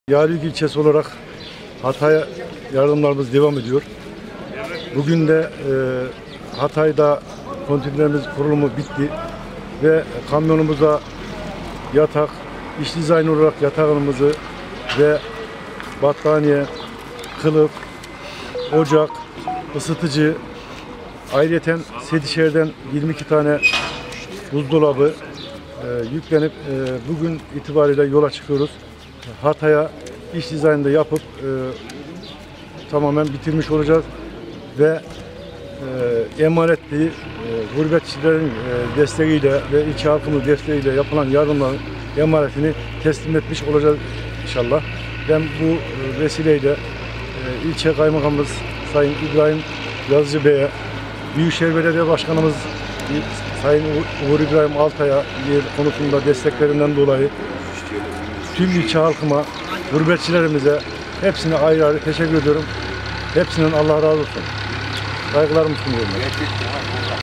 Yalıhüyük'ten giden konteynerler kuruldu iç donanımı için bir kamyon yola çıktı
Konteynerlere yatak buzdolabı ısıtıcı ve  battaniye gönderildi